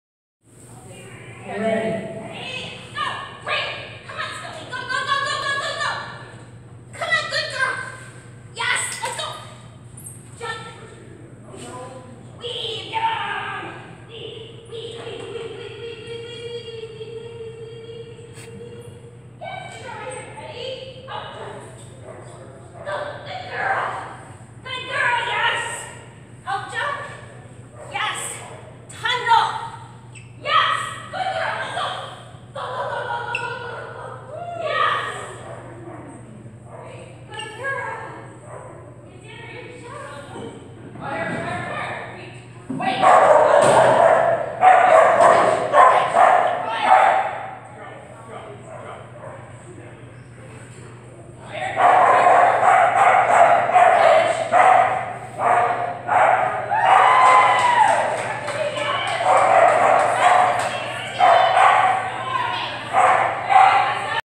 Scully finishing her ADCH
Scully & Fire running Master Pairs, and finishing her ADCH